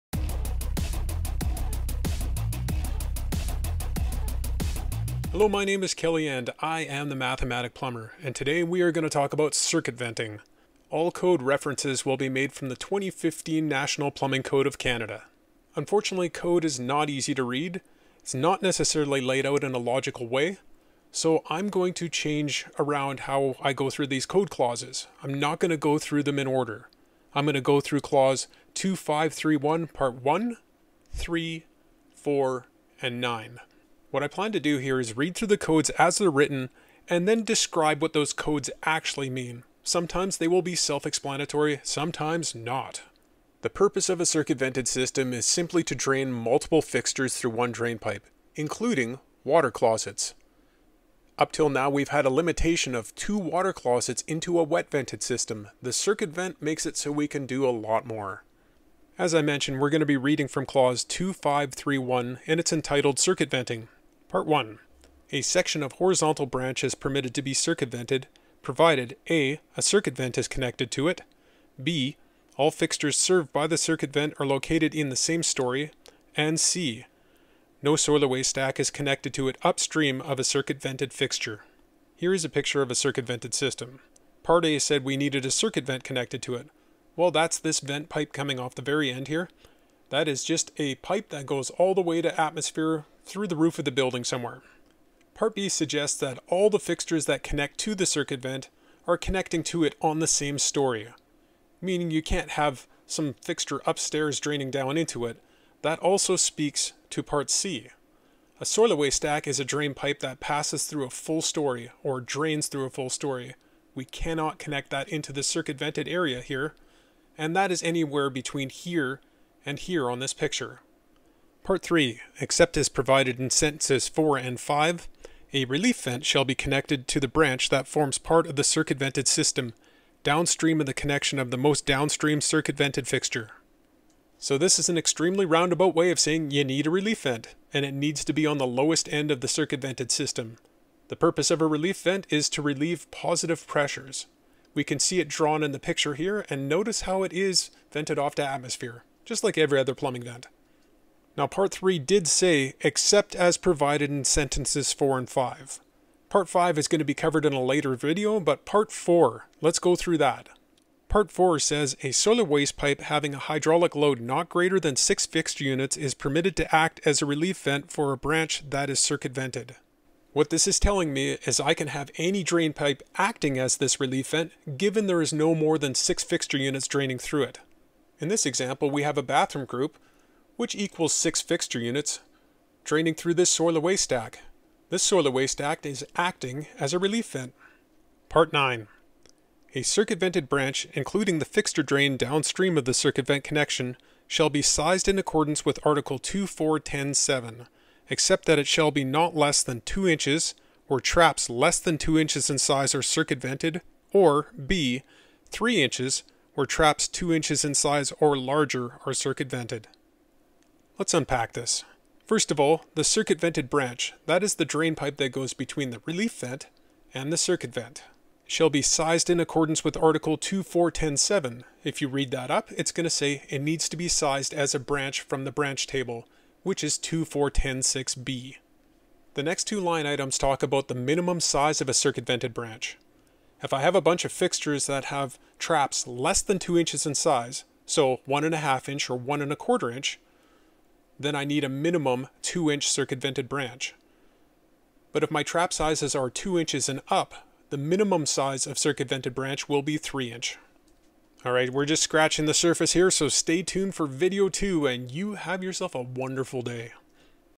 Circuit Vent Part: 1
Welcome to the wonderful world of Canadian Plumbing Code. This goes through Clauses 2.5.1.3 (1, 3, 4 and 9)